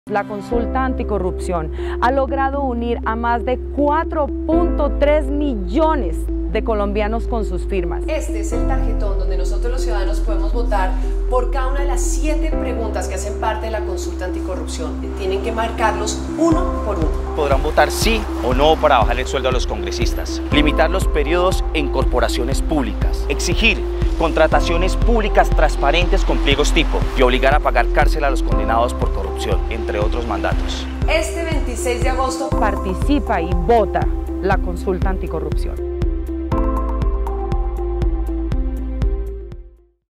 Este 26 de agosto ¡Participa y vota en la Consulta Anticorrupción!
Este 26 de agosto prepárate para votar 7VecesSí en la ConsultaAnticorrupción.